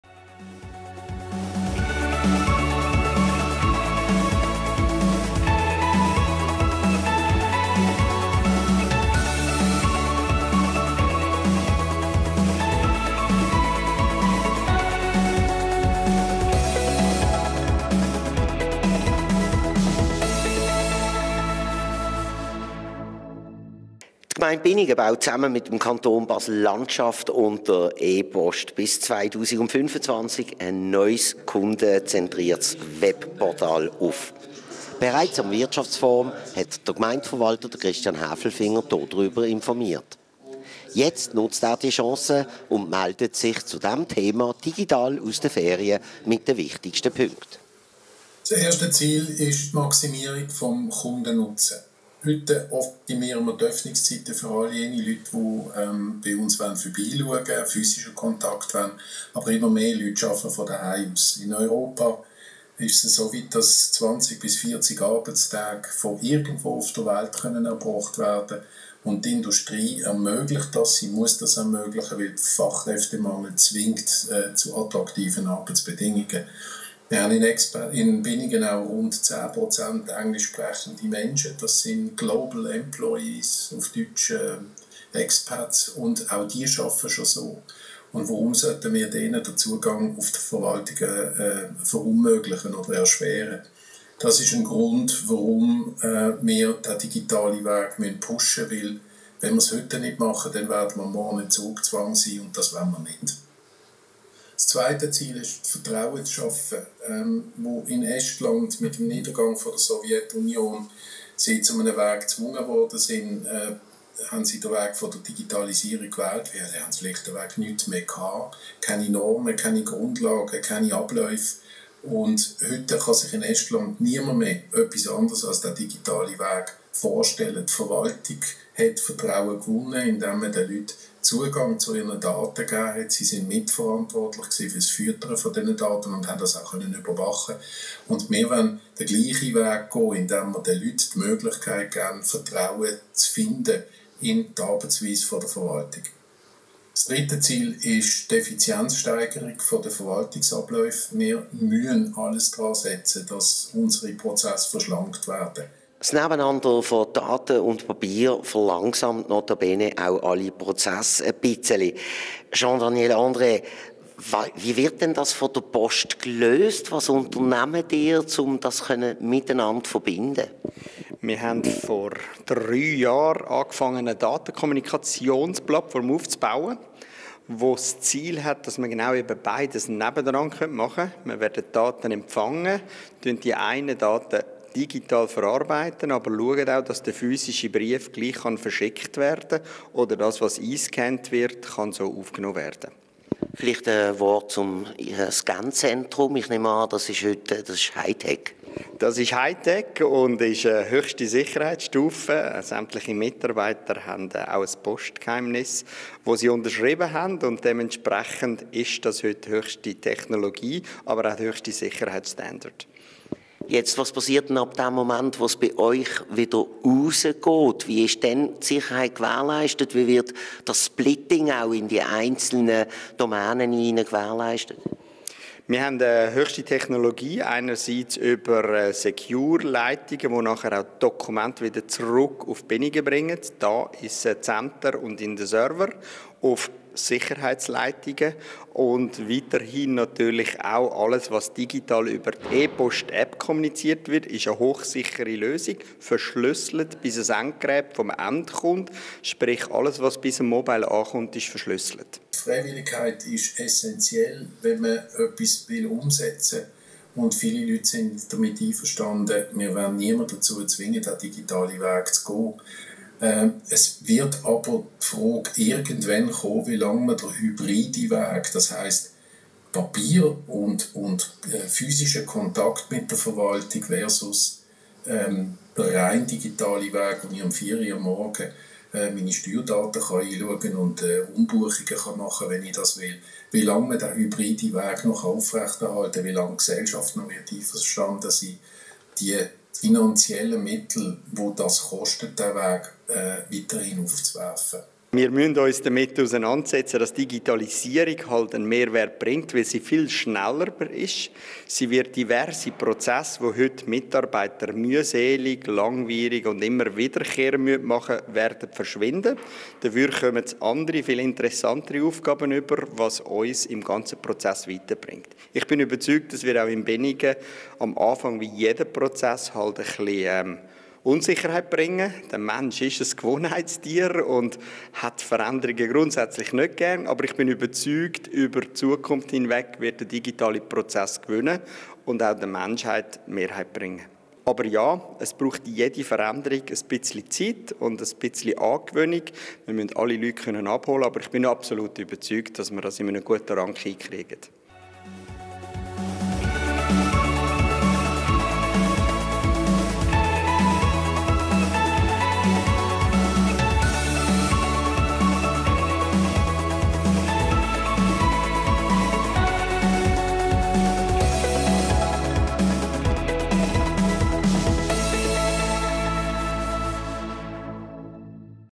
Gemeinde-TV Binningen: Binningen baut ein digitales Kundinnen-/Kunden-Portal
Die Gemeinde Binningen will den Einwohnerinnen und Einwohner den besten Service bieten. Deshalb baut sie zusammen mit dem Kanton Basel Landschaft und der Post von 2023 bis 2025 ein neues kundenzentriertes Webportal aus. Für effiziente Abläufe arbeitet die Gemeinde mit ePost zusammen.